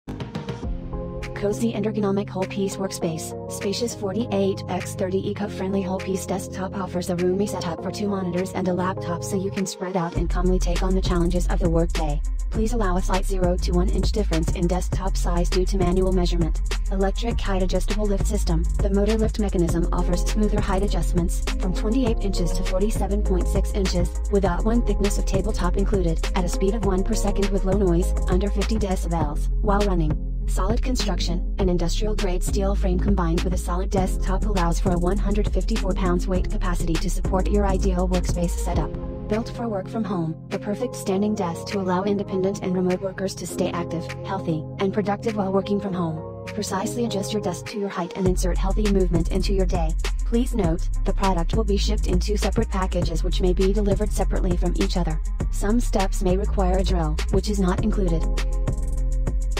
Electric Standing Desk Adjustable Height Desk from Amazon
Flexispot EC1 Electric White Standing Desk Adjustable Height Desk, 48 x 30 Inches Whole Piece Board Sit Stand Desk Home Office Workstation Stand up Desk (White Frame + 48 in White Top)